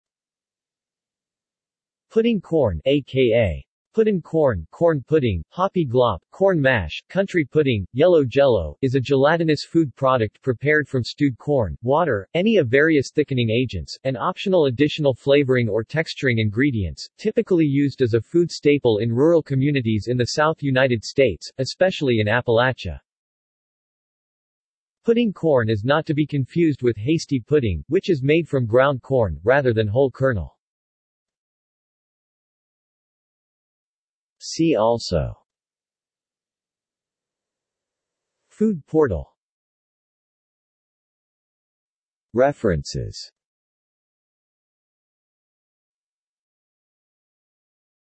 Pudding corn | Wikipedia audio article
This is an audio version of the Wikipedia Article:
Pudding corn

SUMMARY
Pudding corn (a.k.a. Puddin' Corn, Corn Pudding, Hoppy Glop, Corn Mash, Country Pudding, Yello Jello) is a gelatinous food product prepared from stewed corn, water, any of various thickening agents, and optional additional flavoring or texturing ingredients, typically used as a food staple in rural communities in the South United States, especially in Appalachia.
Pudding corn is not to be confused with hasty pudding, which is made from ground corn, rather than whole kernel.